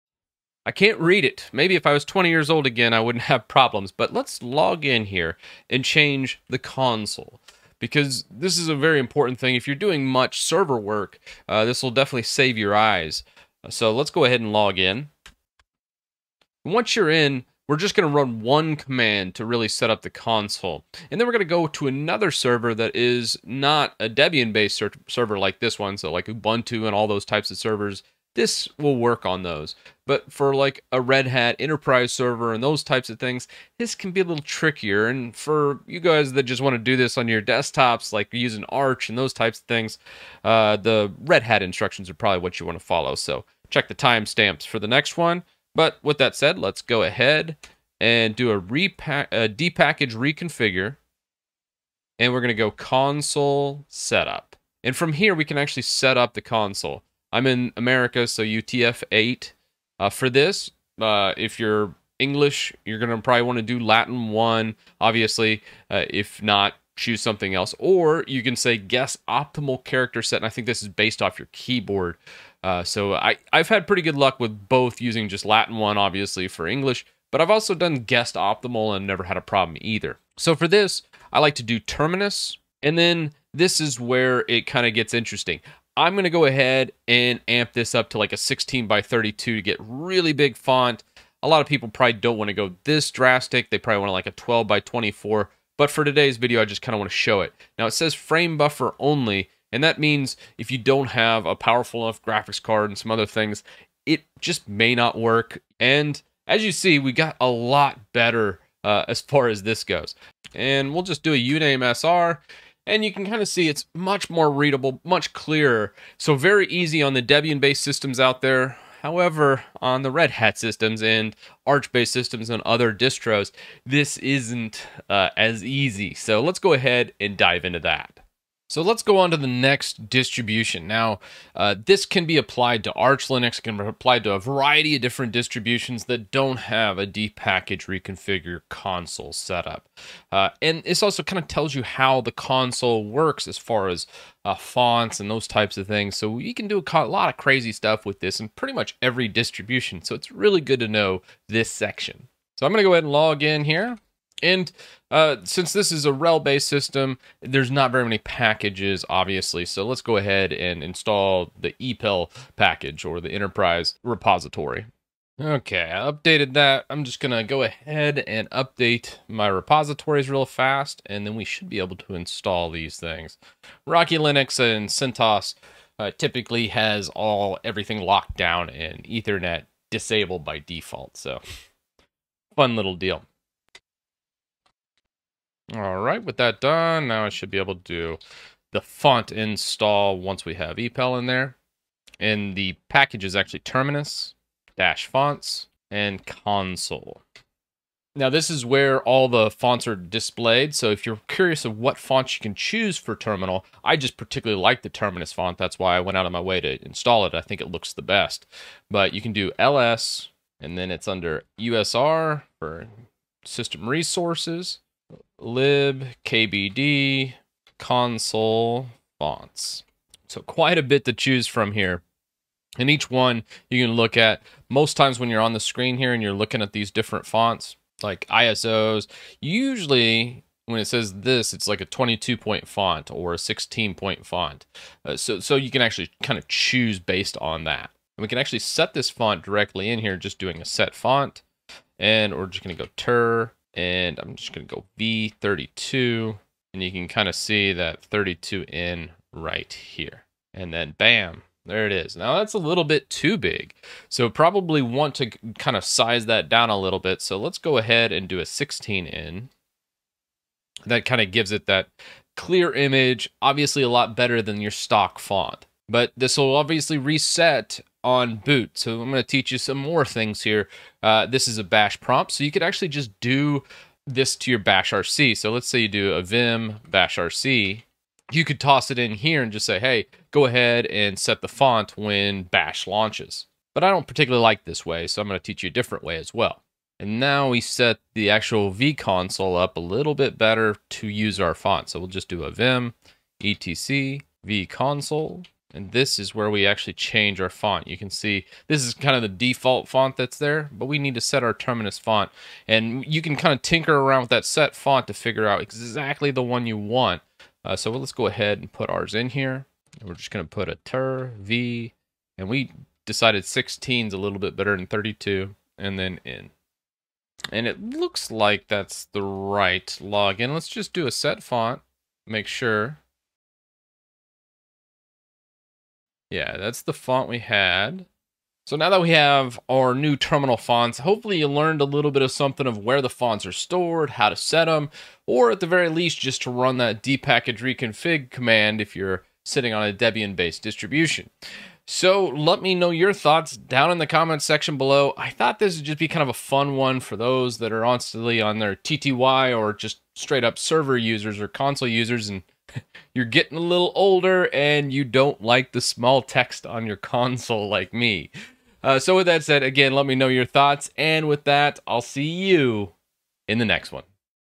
Set Console Font
Set the console font in ANY Linux install. This can be Debian, Arch, or ANYTHING!

Timestamps:
00:00 Debian Install
02:08 Installing on OTHER Linux Distributions .